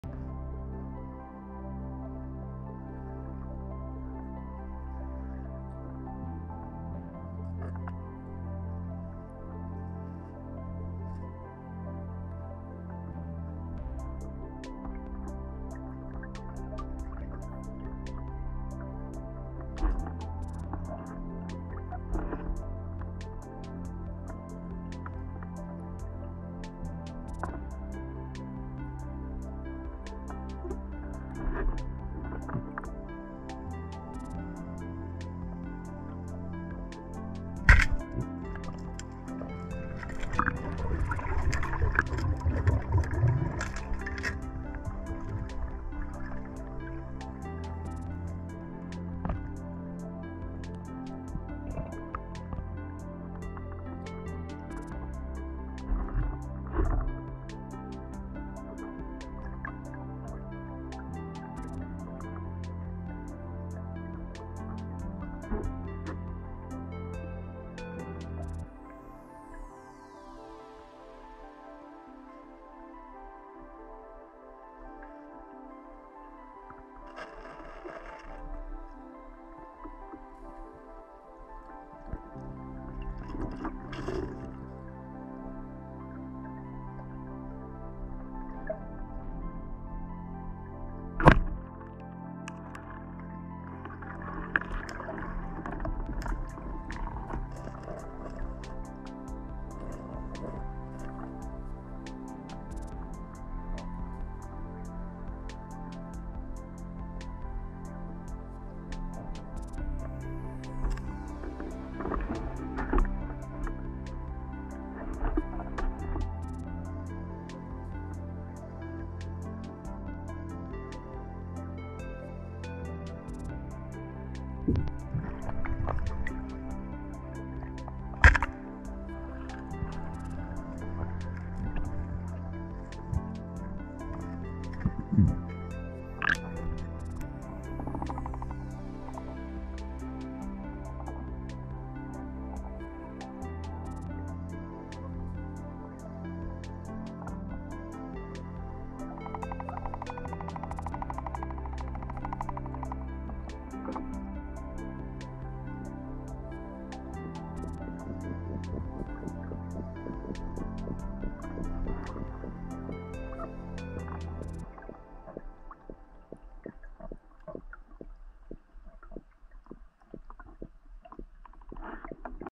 Atlantic Wolffish, Norway
While hunting for cod in Moldefjord on the west coast of Norway, I came across a couple of beautiful Atlantic Wolffish (catfish). The cod were of course invited home for dinner, but I left the Wolffish alone this time.
All recordings were made with Gopro Hero 7 Black.